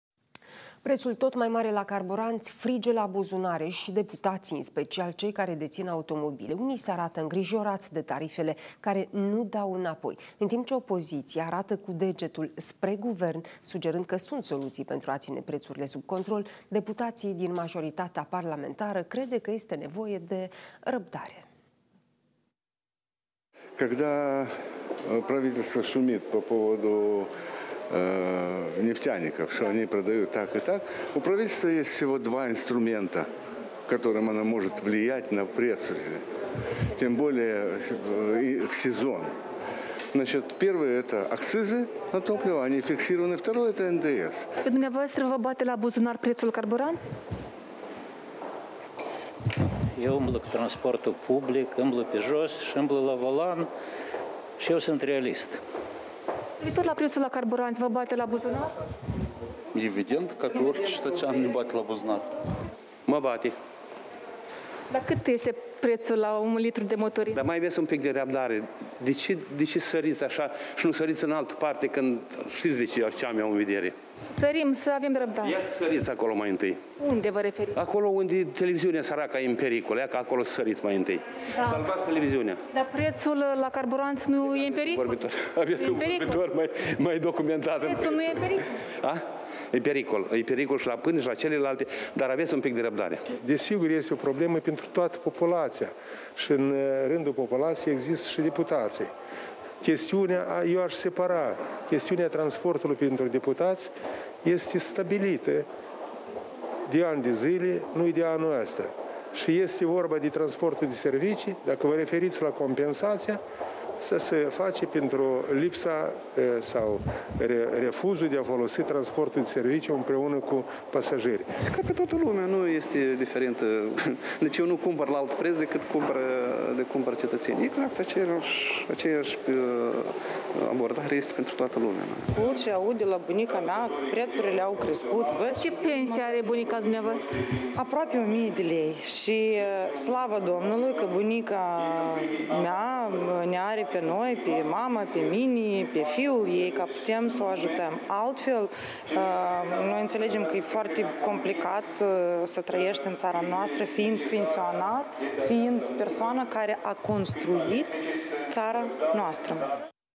Deputații, afectați de scumpiri
Prețul tot mai mare la carburanți frige la buzunare și deputații, în special cei care dețin automobile. Unii se arată îngrijorați de tarifele care nu dau înapoi. În timp ce opoziția arată cu degetul spre Guvern, sugerând că sunt soluții pentru a ține prețurile sub control, deputații din majoritatea parlamentară crede că este nevoie de răbdare.